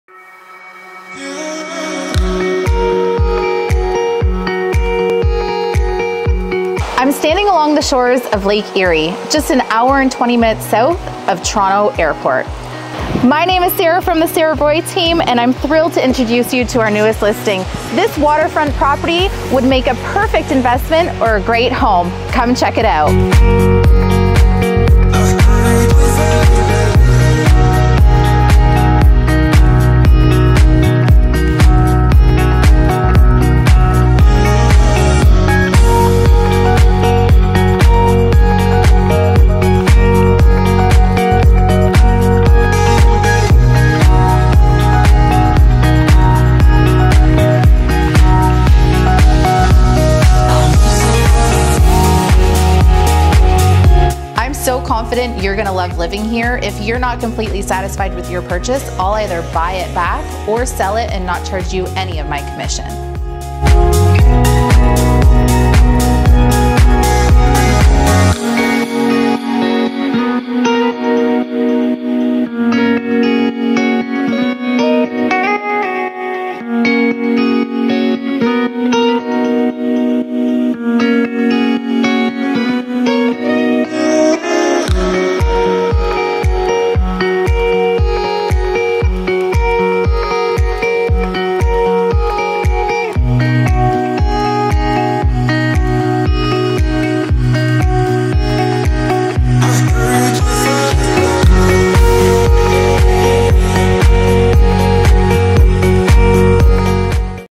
Lake Erie Waterfront Home For Sale
JUST LISTED 1556 Lakeshore Road Selkirk, Ontario

NO DISAPPOINTMENTS HERE!!! Immaculately maintained inside & out and lived in year round for many years. this Lake Erie beach house is what dreams are made of!

Gorgeous custom kitchen with quartz countertops. A living room with bamboo floors, full of sunshine from the sliding glass doors which lead to a covered deck and your own beach! Cozy woodstove for cool evenings and winter snuggling. Main floor stackable laundry in updated bathroom. 2nd story has additional bedrooms/hobby rooms for the kids!

There is a boathouse/workshop doubling as a "man cave or she shed", concrete driveway, decks and patios offering lots of outdoor space for entertaining. Picture enjoying a glass of wine, soaking in your hot tub listening to the waves roll in.

Loads of updates including a new concrete break wall (2020), roof shingles (2016), furnace (2020), central air (2017), replacement vinyl windows throughout, cistern/holding tank (2001) both 2000 gallons. New survey done in 2016.

Nothing to do but plan the family get togethers and bring the towels & sunscreen! Make this summer YOUR summer in your very own lake house!

Presented by Sara Roy, Realtor